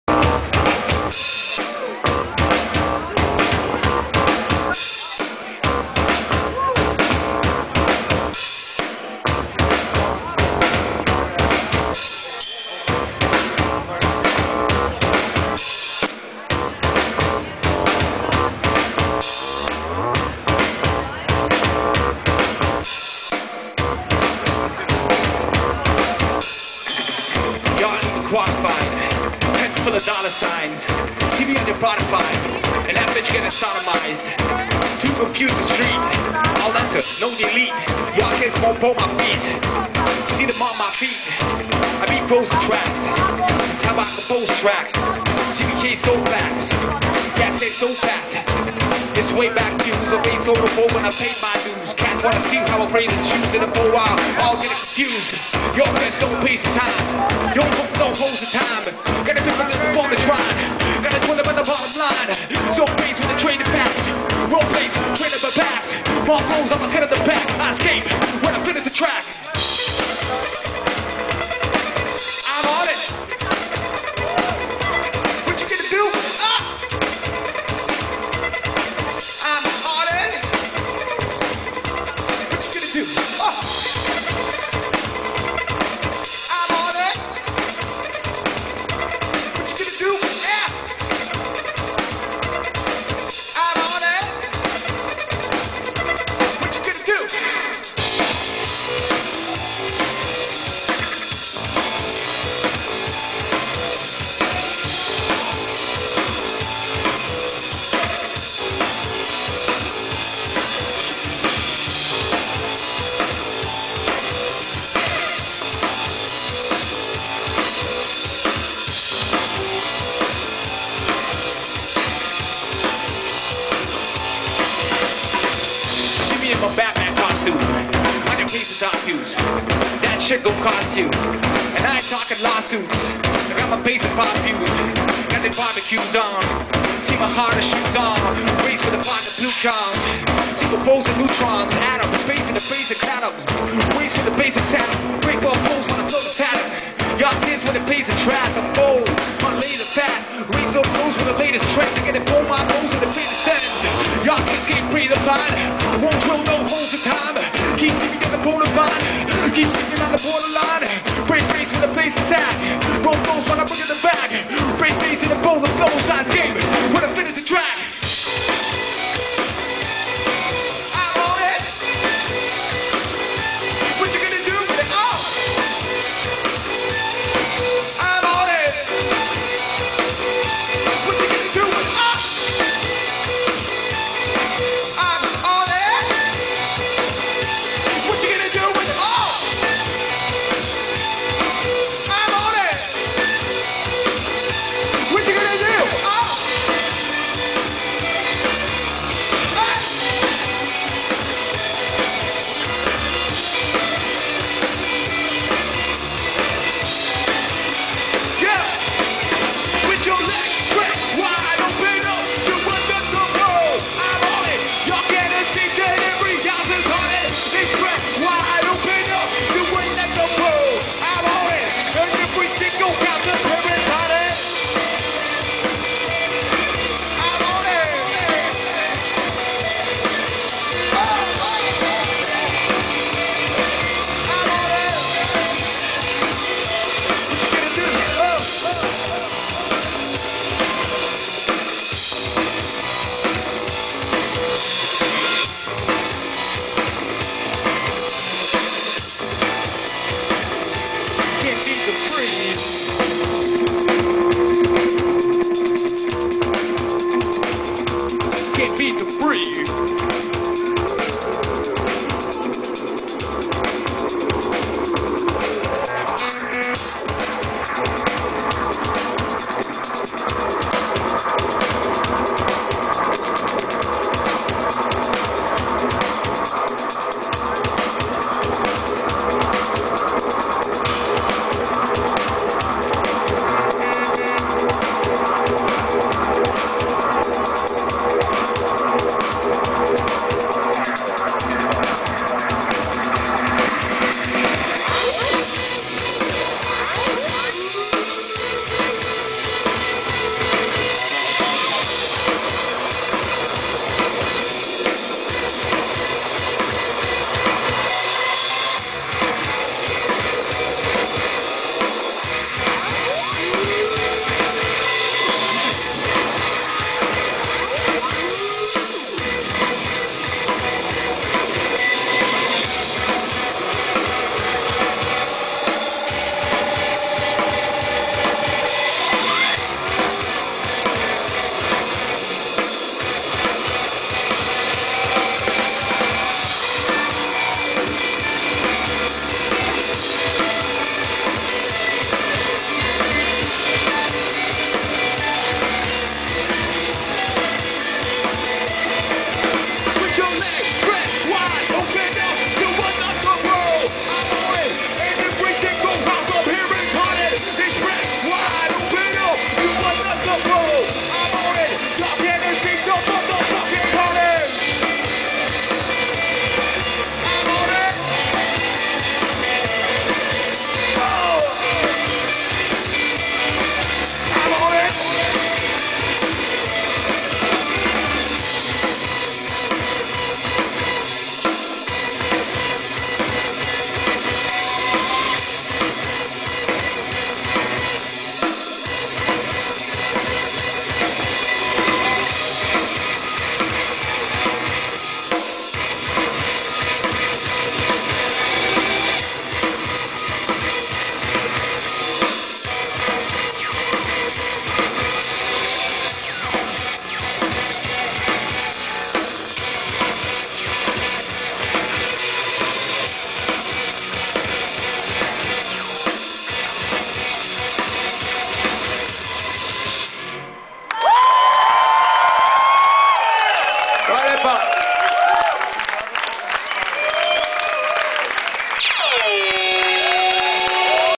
MOVEKNOWLEDGEMENT @ Bikofe, Ljubljana 2013
Performed live on 22th Augustl 2013 at Bikofe, Ljubljana. The copyright rights are of their owners. This is a non-profit fan video.